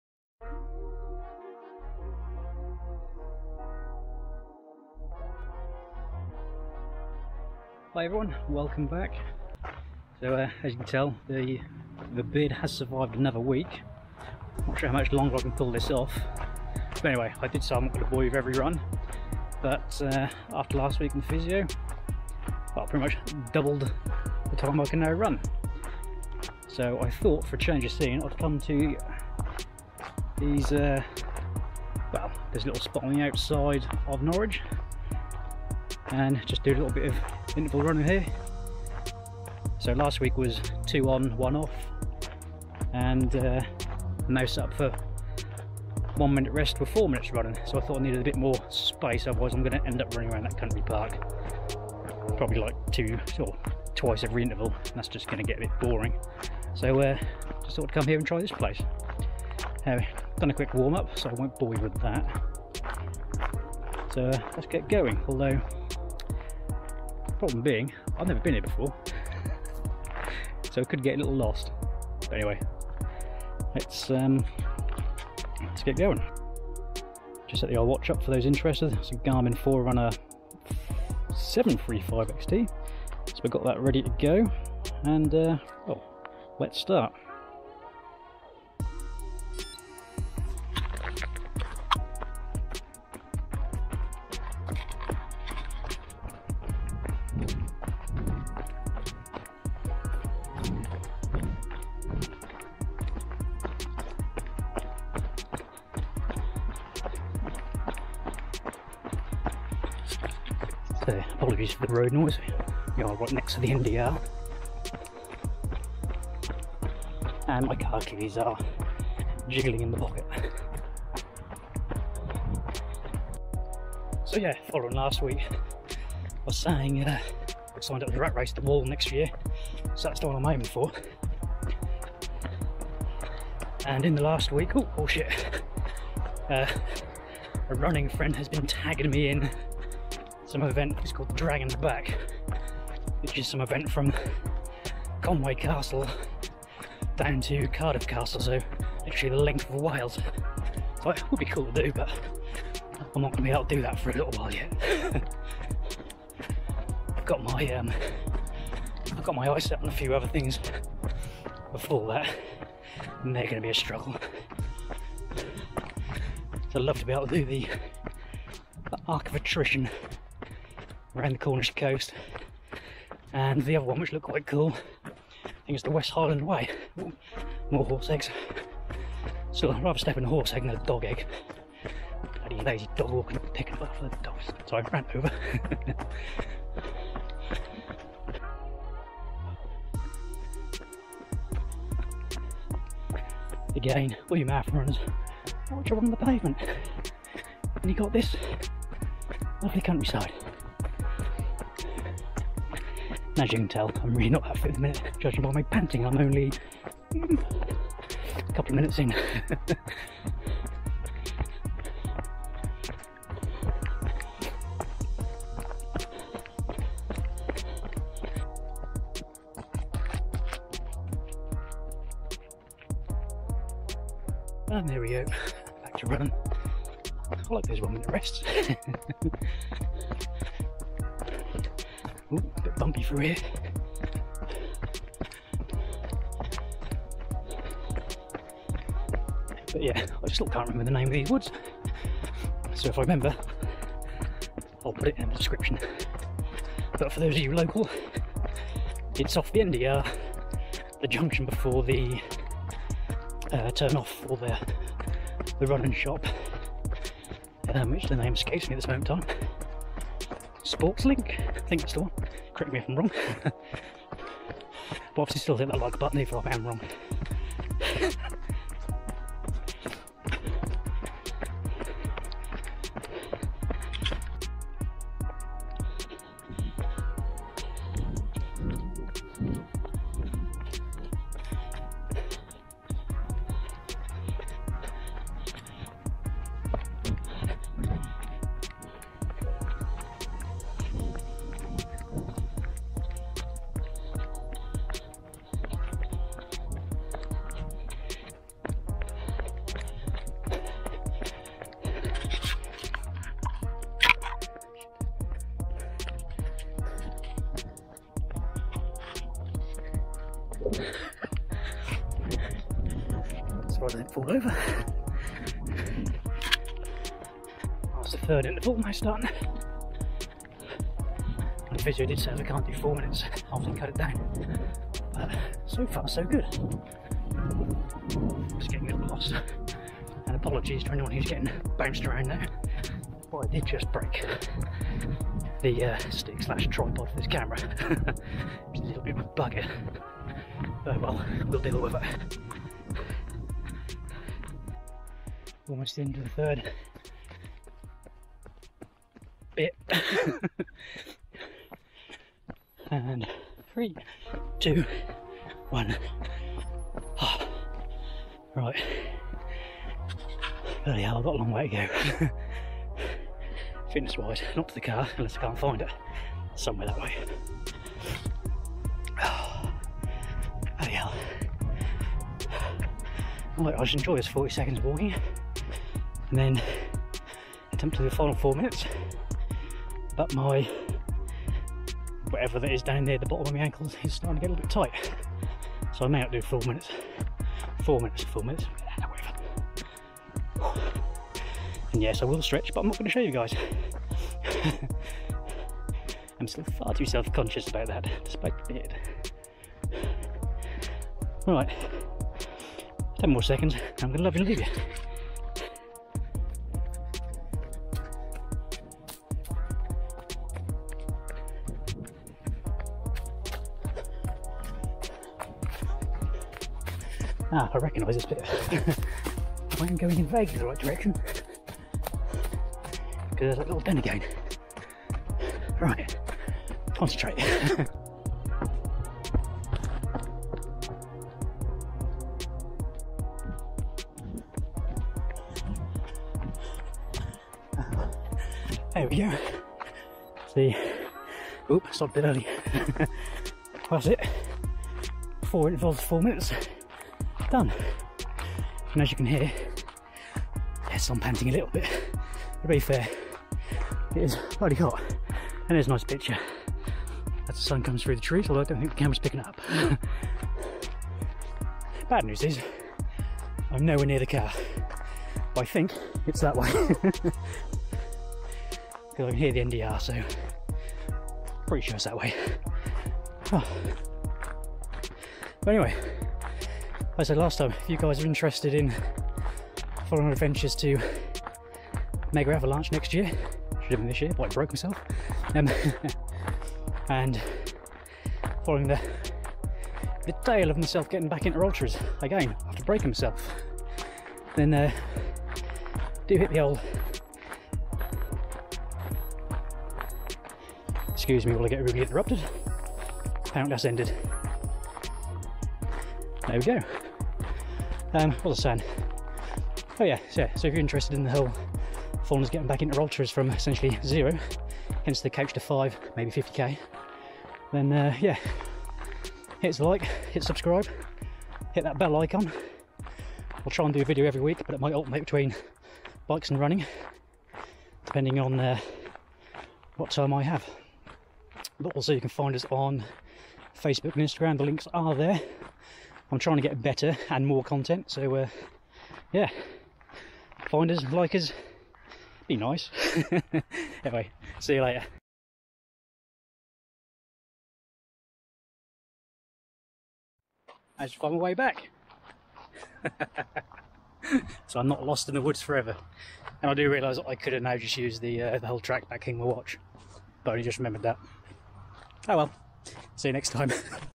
Back on it! Couch to 5 or 50K, week 2.
After a week of gentle interval runs of 2 minutes on with 1 minutes rest, I've up the game to 4 minute intervals. It may only be small gains but compared to where i was a few weeks ago following injury, I'm more than happy with the progress!

For a change of scene I decided to visit 'The Mystical Woods' on he outskirts of Norwich for this running session.